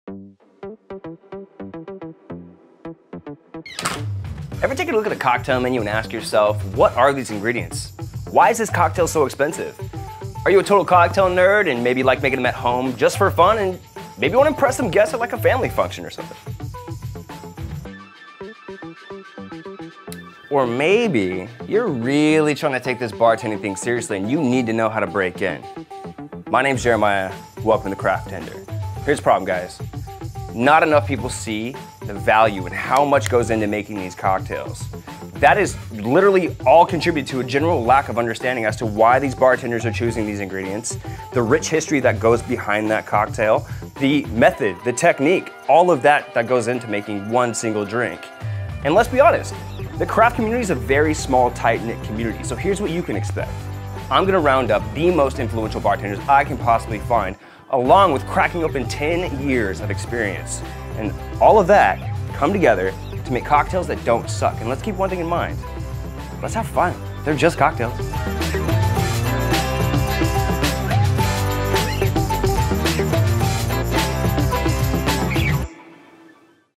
Craft Cocktails That Don't Suck! - Welcome to the CraftTender YouTube Channel
Ever wonder why a craftcocktail costs so much? Looking to make cocktails at home or get into the mysterious world of craft bartending?

CraftTender is here to guide you through everything you need to know!